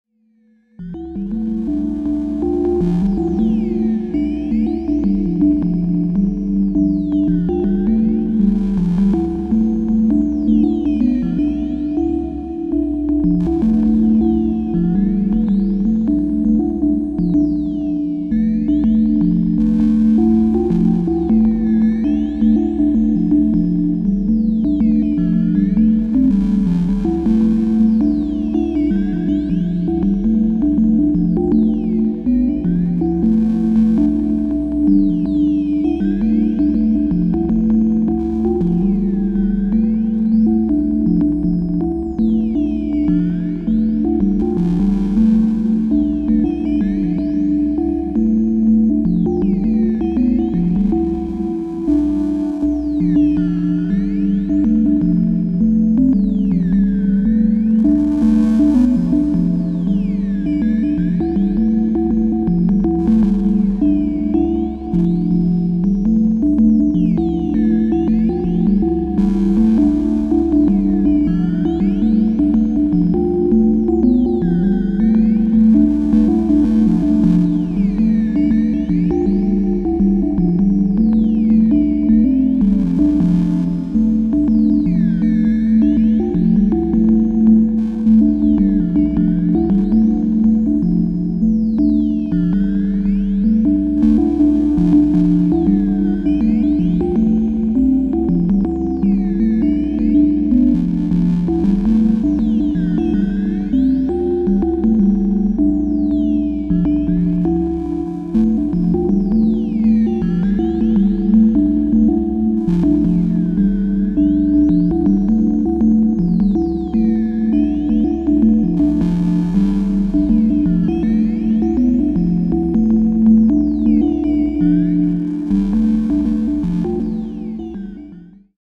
2023-11-12 - failed to remove high pitch sound
Noticed the soft stacked flares from the left leds in the anamorphic optic? 😊
ZCam E2-F6 with Sirui 50 mm T2.9 1.6x FF Anamorphic + Black mist filter. Film grains added in DR.
Only sound source is the Polygogo, filters, reverb and echo.